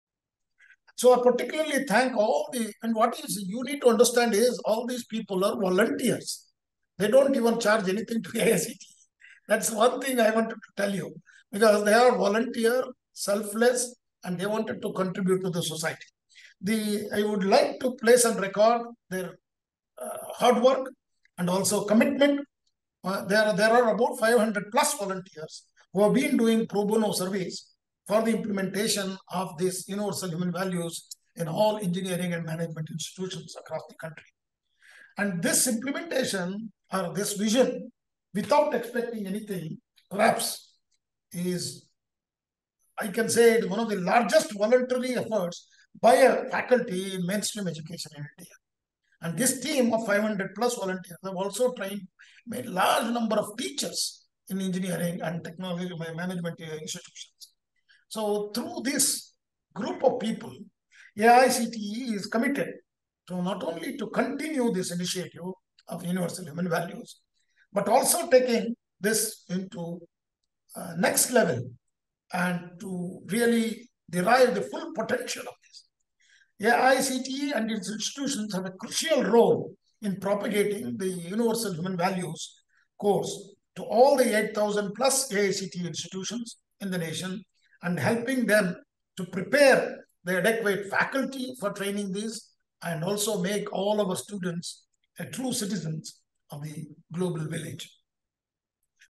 UHV Team Volunteer Effort and AICTE's Commitment to Human Values
Volunteer UHV Team - perhaps the largest volunteer effort by a faculty team in the Nation
AICTE Commitment to take UHV to the next level, to all AICTE institutions